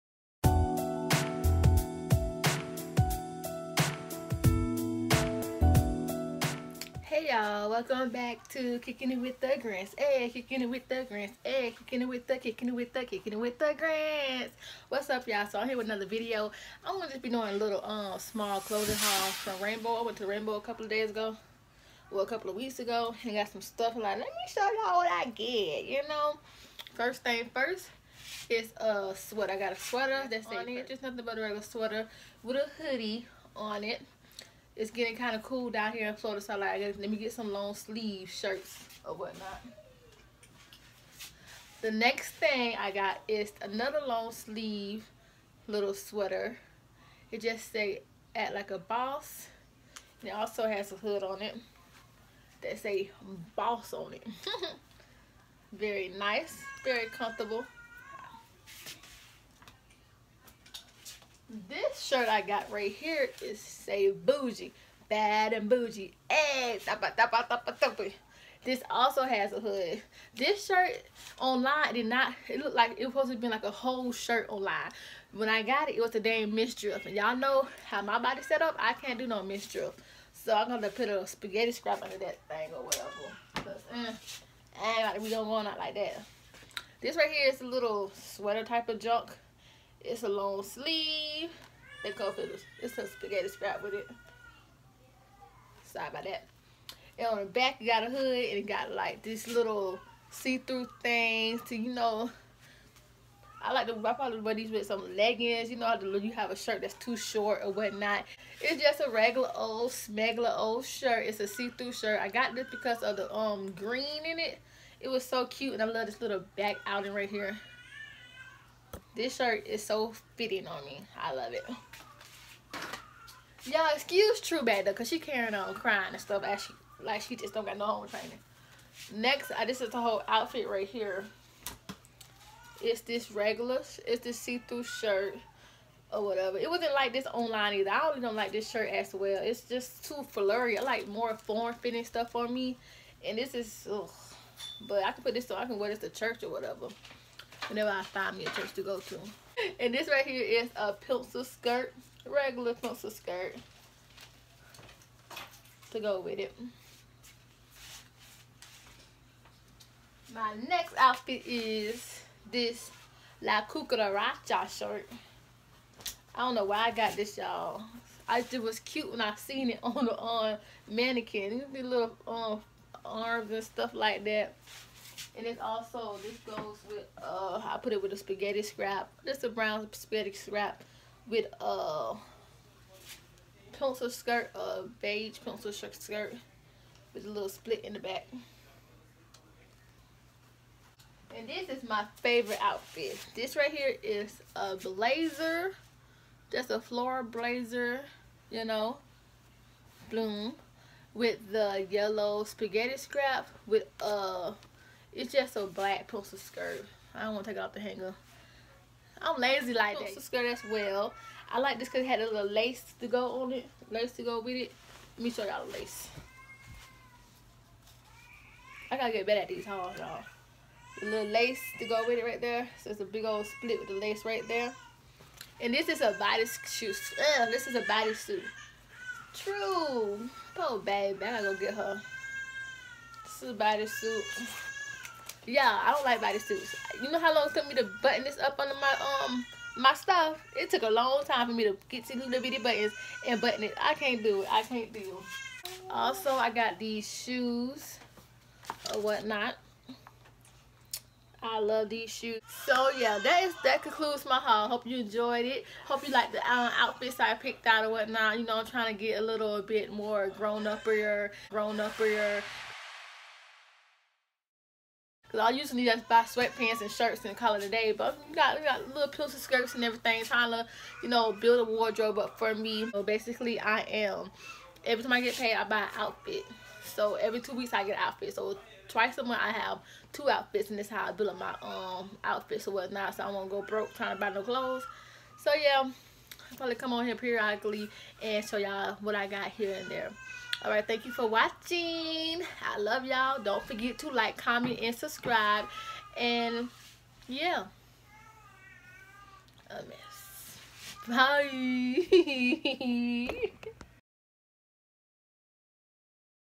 Rainbow Clothing Haul+Try On
Hey y'all just wanted to share a couple outfits with u guys. I really love Rainbow Stores there very cheap and cute at the same time lol. Y'all should go check them out.